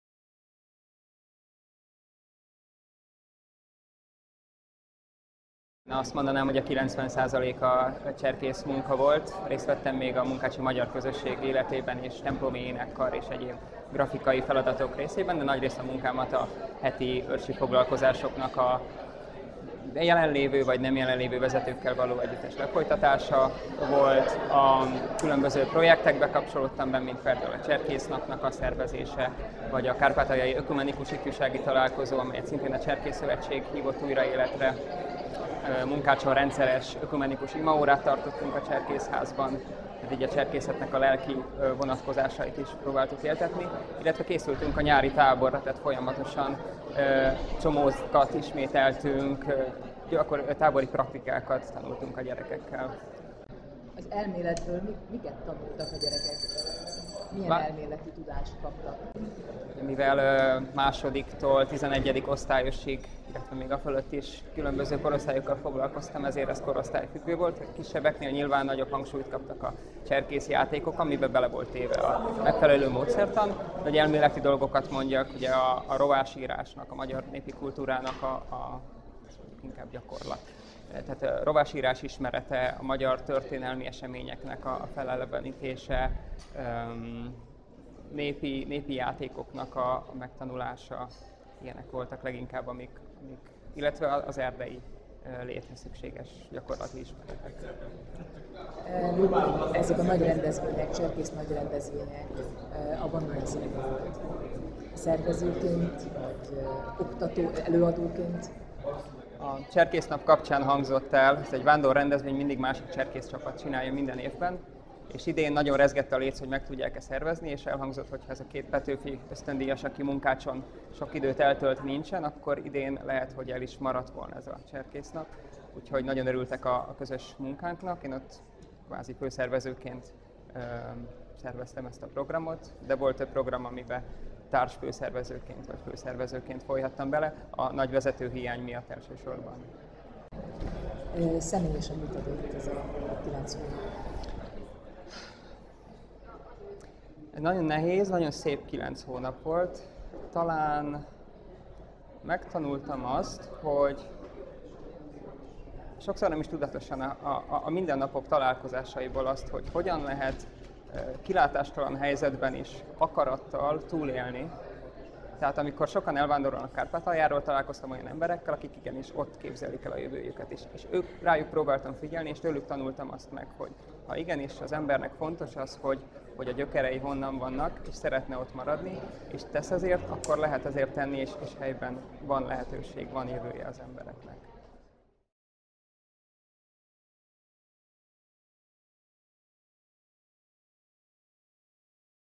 Szolgálatból visszatérve: Kárpátalja
A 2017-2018-as programévben a Nemzetpolitikai Államtitkárság 65ösztöndíjast küldött a szórványba, és 115 ösztöndíjast a diaszpórába abból a célból, hogy értékmegőrző, hagyományteremtő munkájukkal támogassák a külhoni magyarok identitását, hogy erősítsék az anyaország és a hazájuktól elszakadt magyarok kapcsolatát.

A külhonból hazatért ösztöndíjasok mesélnek tapasztalataikról, élményeikről.

Techet Gábor, Beregszász